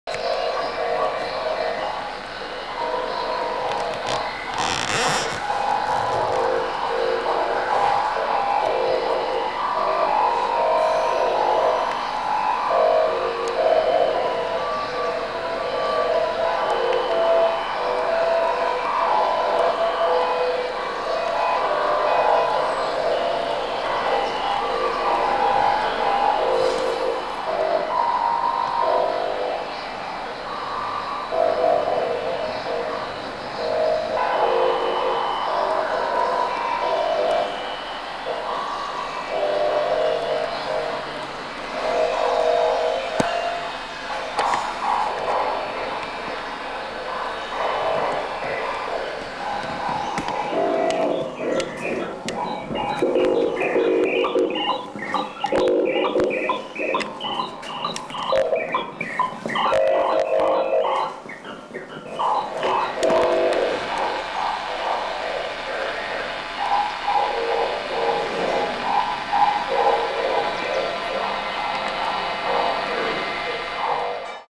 The Sound of meredian Crossings
Site specific Sound Installation involving Sounds from short-wave Radio broadcast
Center of Metamedia, Claster Plasy (Cz) 1995
Different wave ranges of two VHF-(very high frequency) receivers which are equipped with horizontal long-witre antennas This installation has been designed for a monastery in Plasy (Czech Republic). The nave of a former chapel -which had just been restored- was an ideal place to put up the long-wire antennas. Two small parabolic mirrors reflected the sounds they received through small loudspeakers on the front wall and on the back wall of the chapel. The installation was in operation for 24 hours. Because of the shifts in the wave ranges, the sound was constantly changing. The diffusely spreading sounds were inviting spectators to wander around in the chapel directly underneath the long-wire antennas which were installed -in the shape of the cross- in a height of 8 meters.